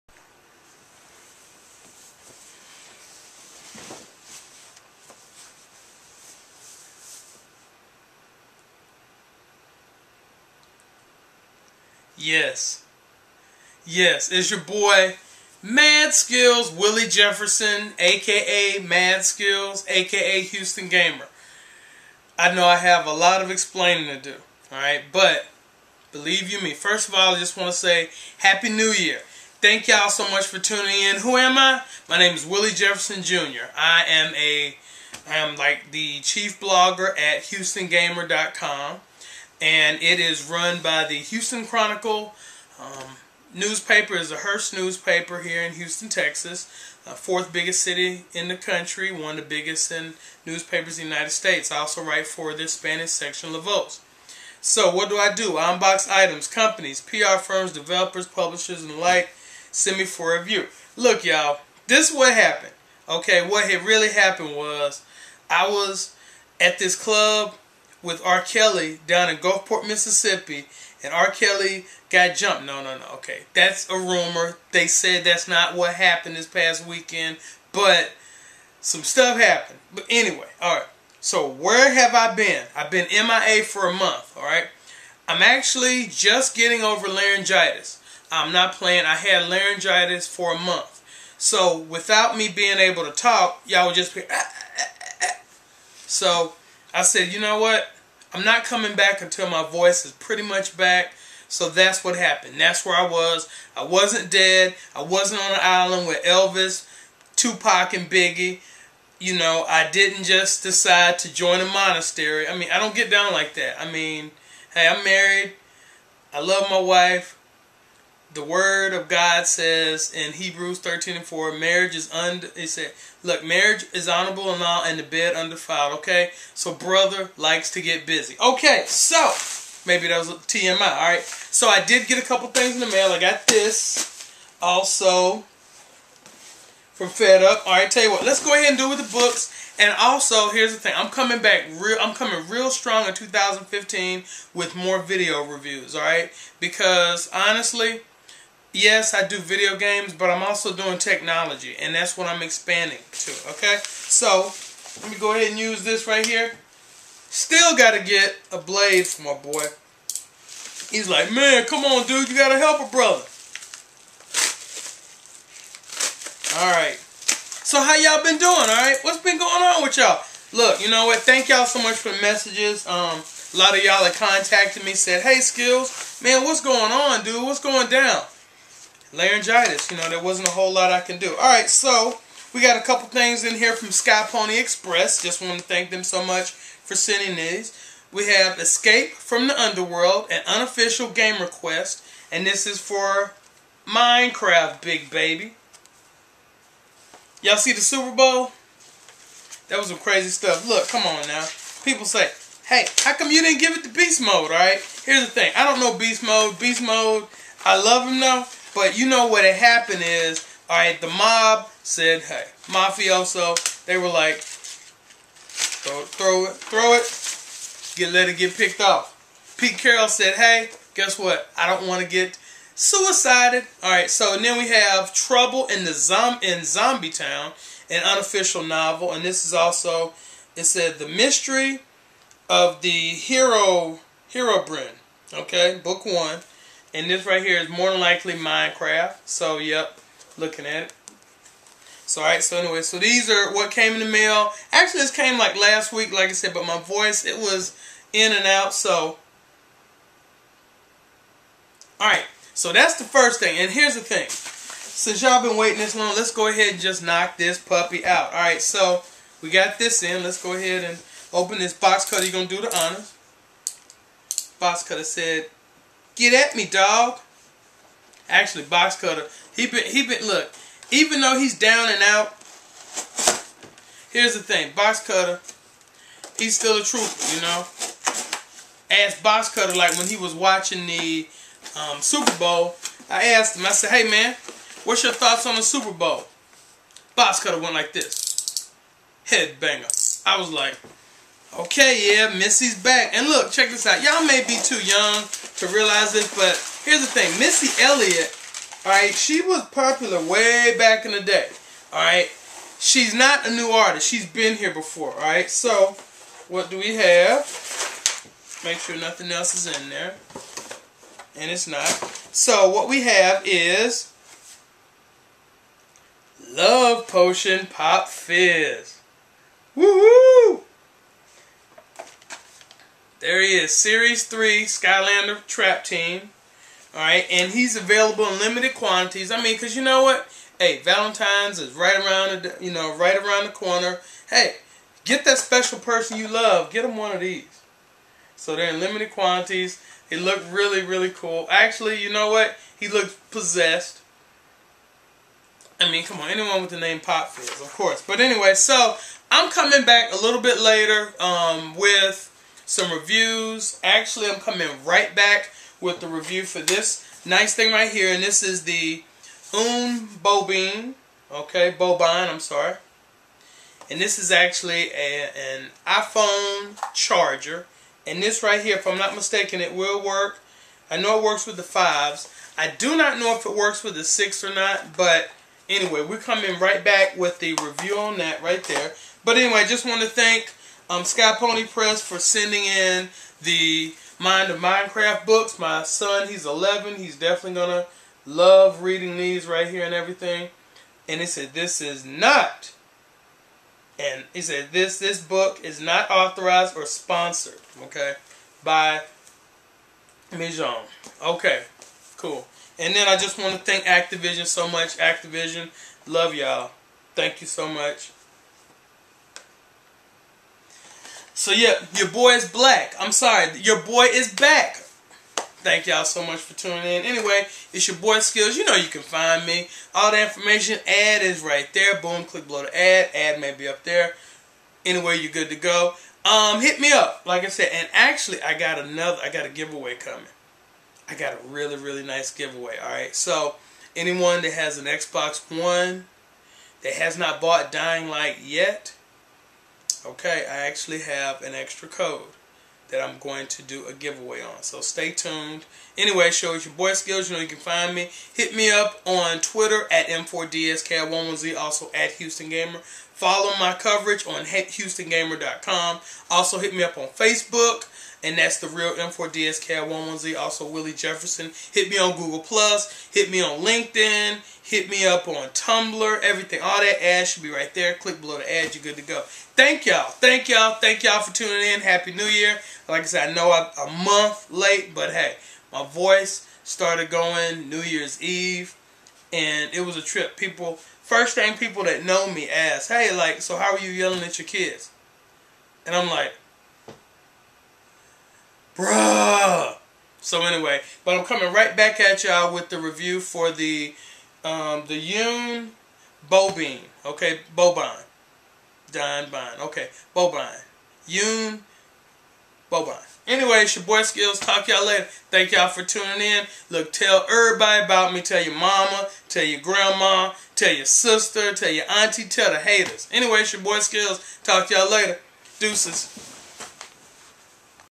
UNBOXING Love Potion Pop Fizz SKYLANDER [HD]
Much thanks to Activision and PMK BNC for the Valentine's Day flavored Skylander Love Potion Pop Fizz! Hold it down!